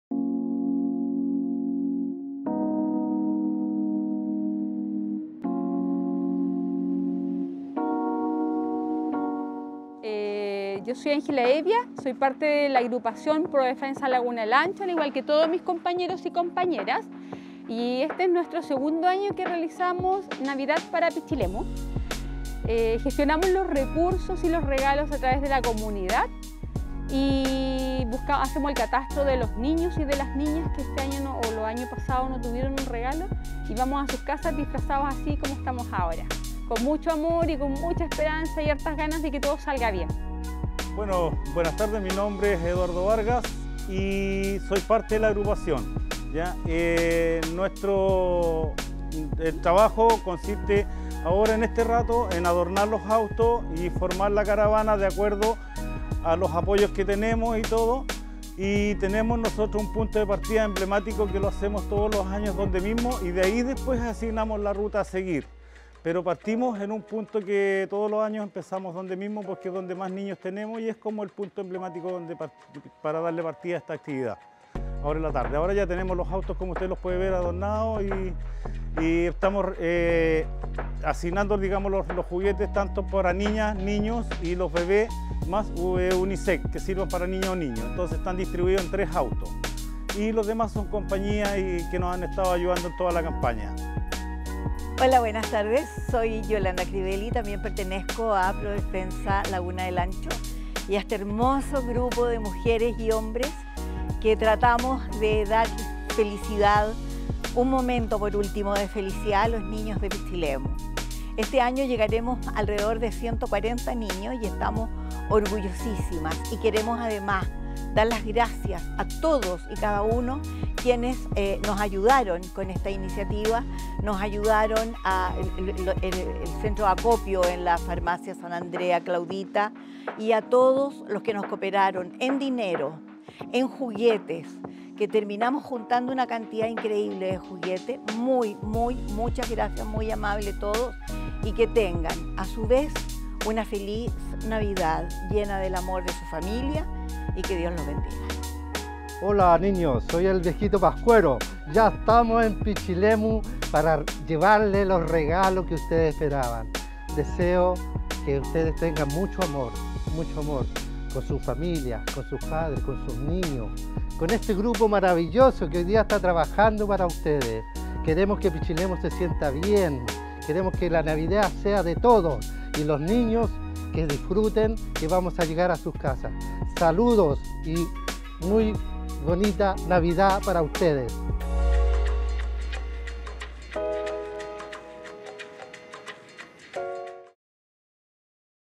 Pro Defensa El Ancho repartió juguetes donados a niños en Navidad
La Agrupación Pro Defensa de la Laguna El Ancho es una de las organizaciones pichileminas que se han unido nuevamente para hacer de esta una feliz navidad. A través de su campaña “Navidad para Pichilemu” se encuentran recolectando donaciones de regalos para niños y niñas de las familias más afectadas por la pandemia.